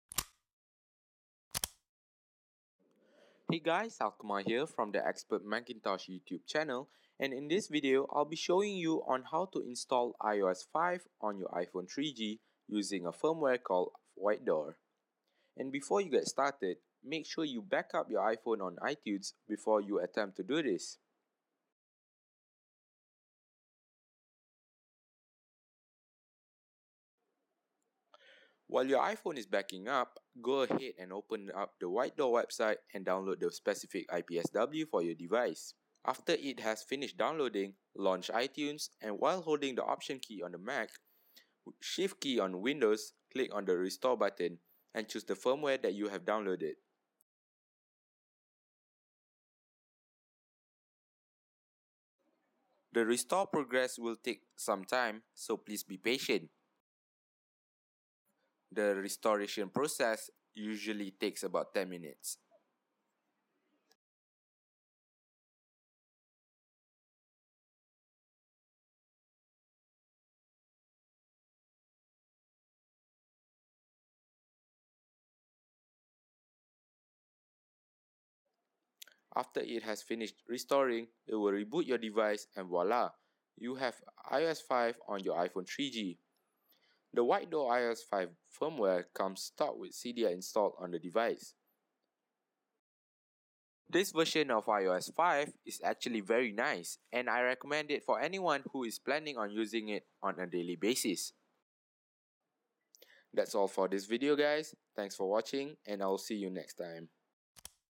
Install iOS 5 On iPhone 3G, 2G, iPod Touch 1G And 2G - WHITED00R
IOS 5 iPod Touch 2G MB Models (Not available for MC models)

Refer to the Whited00r website, for further instructions.